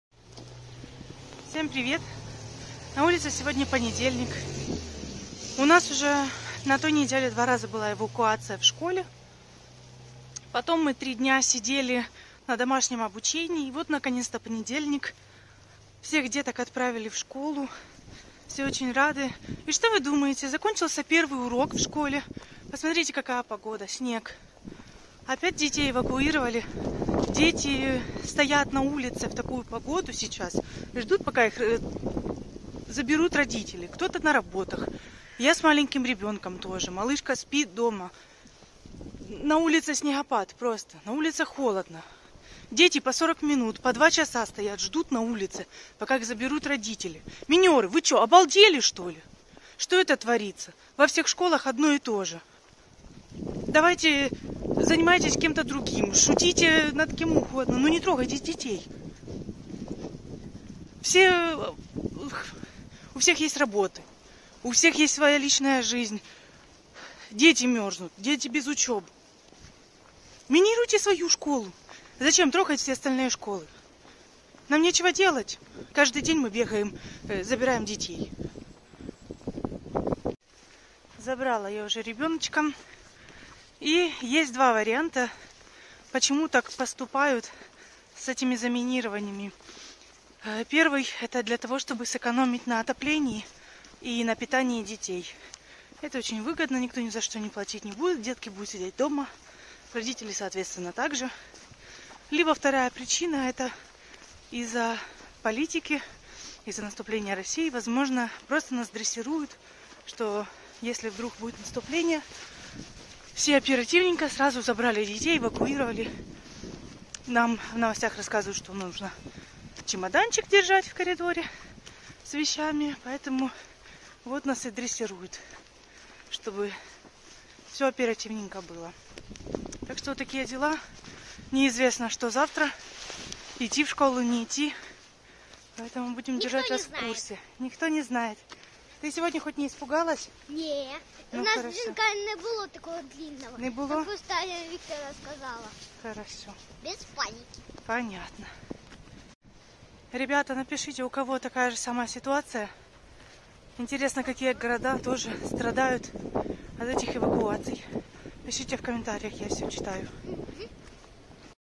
ЗАМИНИРОВАЛИ‼️ Эвакуация со школы Кривой рог‼️ЗАМИНИРОВАЛИ 24 января
Утром 24 января в Кривом Роге снова поступило сообщение о минировании всех школ. В настоящее время проводится эвакуация.

Около 8:00 в полицию перенаправили письмо от неизвестного анонима. Он представился как секретный агент России и пригрозил, что через час все школы Кривого Рога взлетят на воздух.

Напомним, на прошлой неделе из-за постоянных минирований школ их временно перевели на дистанционное обучение. В этот понедельник, 24 января, очное обучение за партами должны были возобновить.